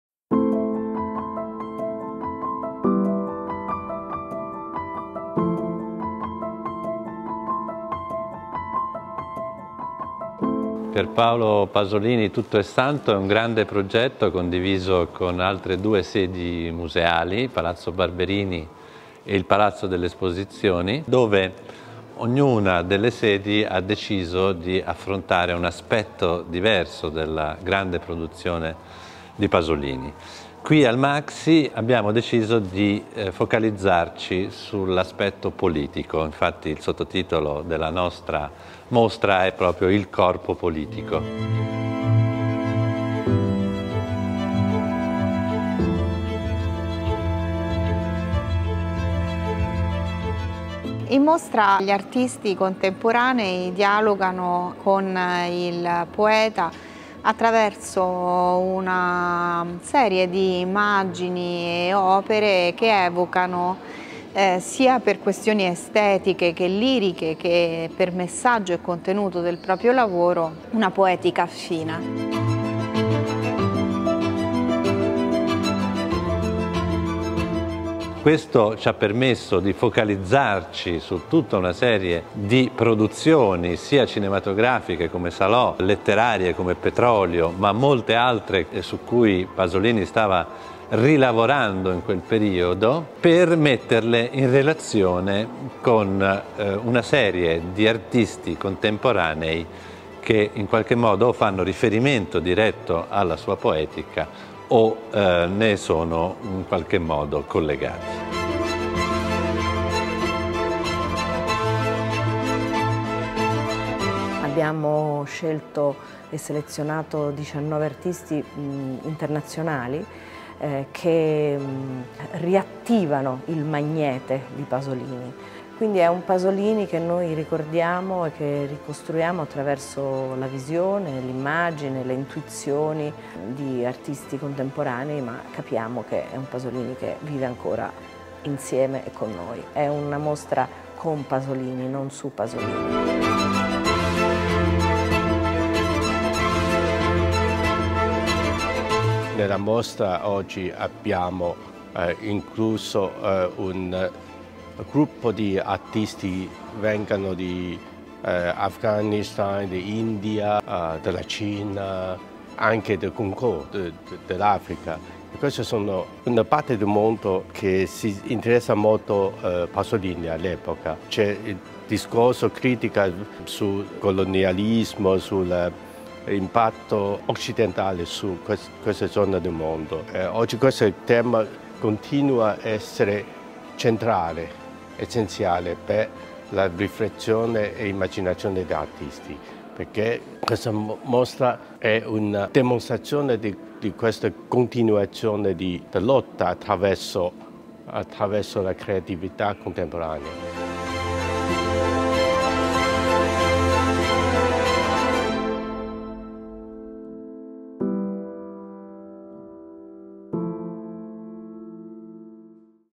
mostre: Pier Paolo Pasolini. Tutto è santo. Il corpo politico
La mostra affronta il tema della sacralità vista da diverse angolature, si snoda secondo tracce differenti ma complementari tra gli spazi espositivi di Palazzo Barberini, il MAXXI e Palazzo delle Esposizioni.

Ricorre nel 2022 il centenario della nascita di Pier Paolo Pasolini, intellettuale e creativo tra i più complessi e rilevanti del XX secolo. Per questa occasione le Gallerie Nazionali Barberini Corsini, La Fondazione MAXXI e l’Azienda Speciale Palaexpo di Roma danno vita ad un progetto coordinato volto a celebrare il poeta nelle tre sedi museali romane.

Tutto è santo è il titolo ispirato all’omonima frase pronunciata dal saggio Chirone nel film Medea (1969), e tratteggia ciò che è considerato sacro per il poeta: il mondo del sottoproletario, arcaico, religioso che è in netto conflitto con l’eroe di un mondo razionale, laico, borghese e neo-capitalista. Al MAXXI la chiave di lettura dell’opera pasoliniana è restituita attraverso le voci di artisti contemporanei, le cui opere evocano l’impegno politico dell’autore e l’analisi dei contenuti sociali ispirati alle sue opere.

La dedizione costante ai temi della vita pubblica, l’osservazione acuta delle dinamiche di potere, hanno fatto di Pasolini un profeta a cui molte generazioni hanno guardato per tracciare le linee guida della propria ricerca. La genuinità del volgo che diventa sacralità, la scomparsa delle periferie, gli effetti di un consumo mediatico massiccio sul grande pubblico, i grandi poteri letti come forze disgreganti del nostro presente, la voce dell’artista come atto di protesta e profezia sono i temi che articolano le sezioni pensate per la mostra.
16 novembre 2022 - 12 marzo 2023
galleria 3
a cura di Hou Hanru, Bartolomeo Pietromarchi, Giulia Ferracci